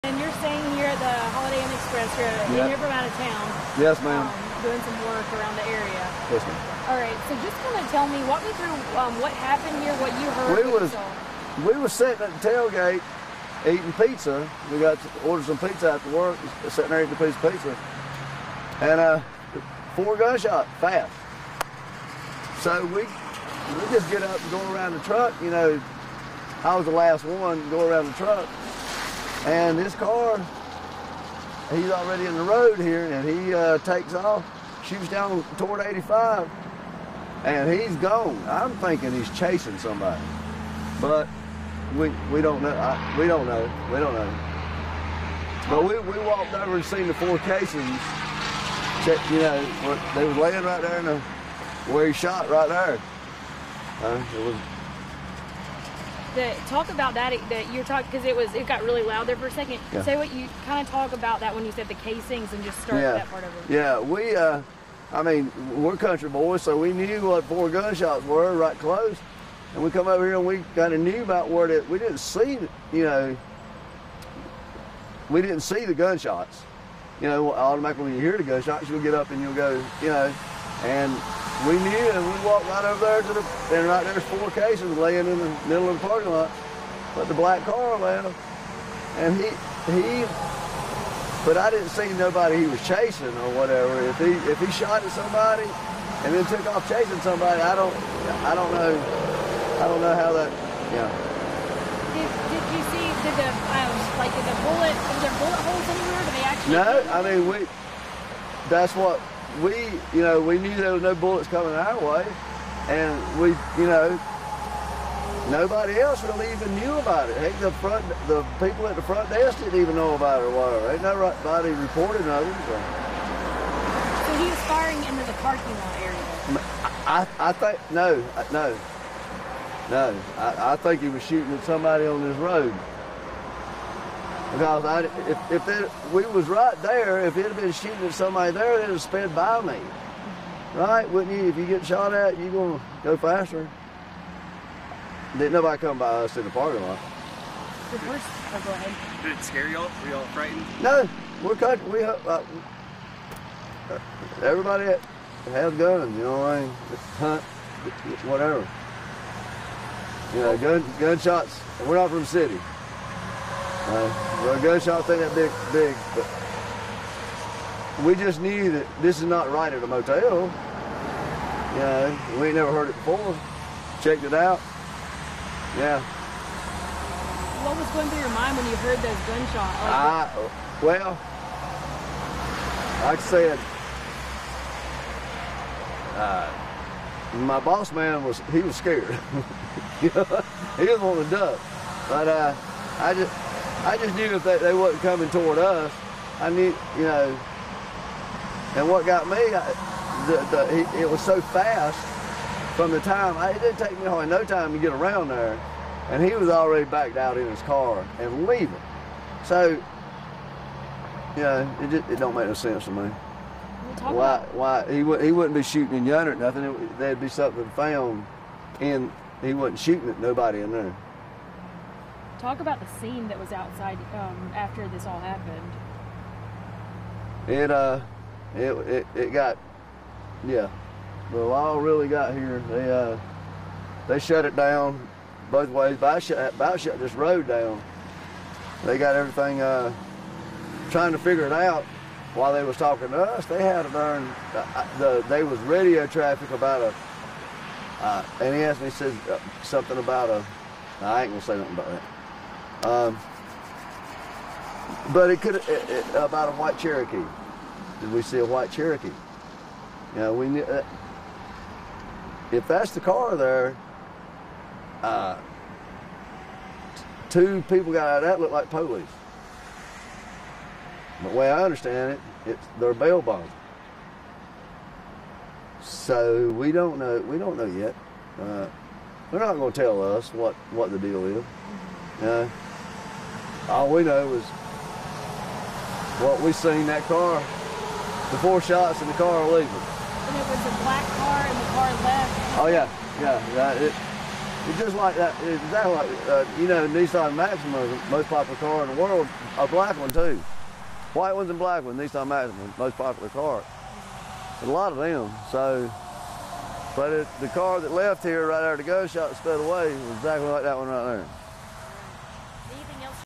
Witness heard four gunshots outside Spartanburg Co. hotel
David Brooks said he was sitting on a tailgate eating pizza with co-workers when four gunshots rang out outside the Holiday Inn Express on Sha Lane (Nov. 7, 2019)